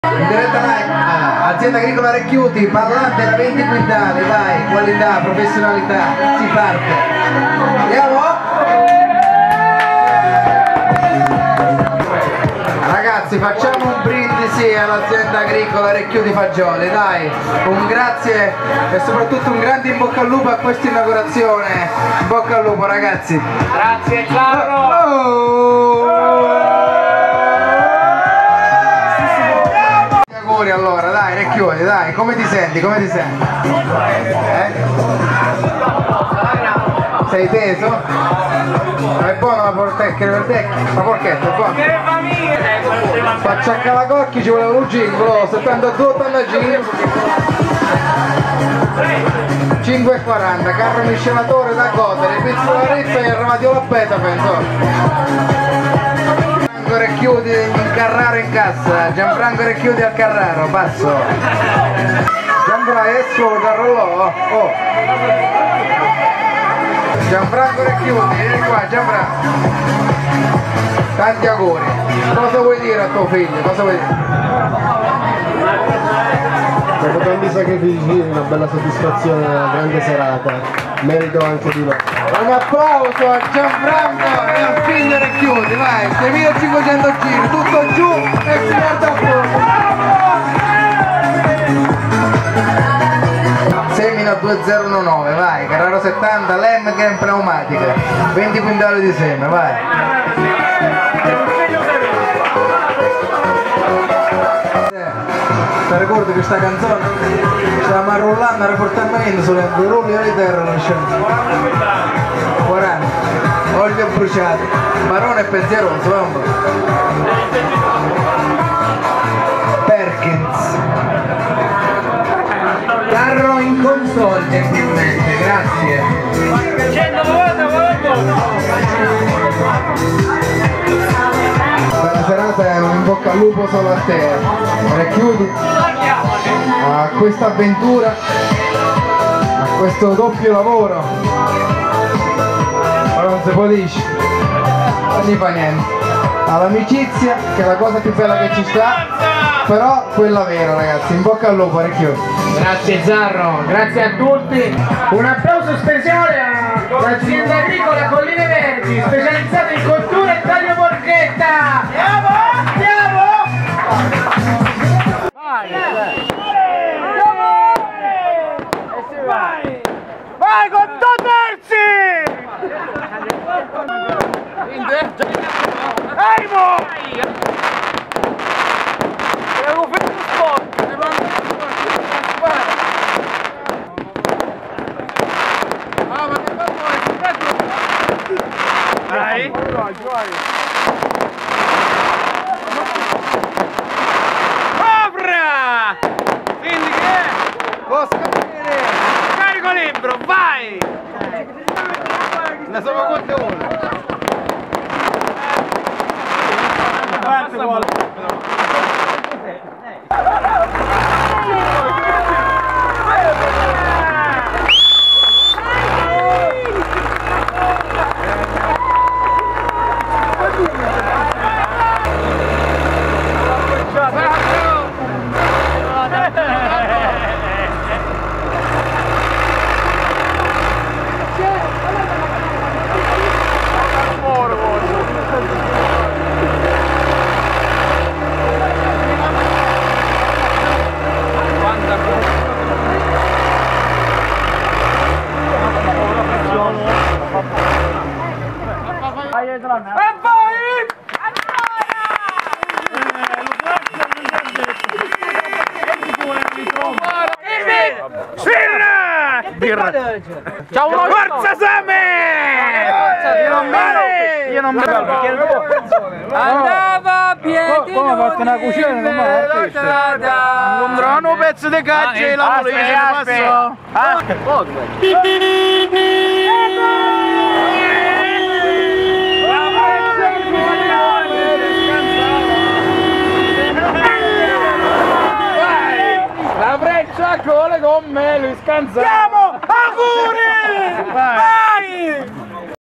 [Carraro 70] Azienda Agricola Fagioli-Recchiuti
Inaugurazione nuova stalla Azienda Agricola Fagioli-Recchiuti presso Notaresco (Te) con la compagnia di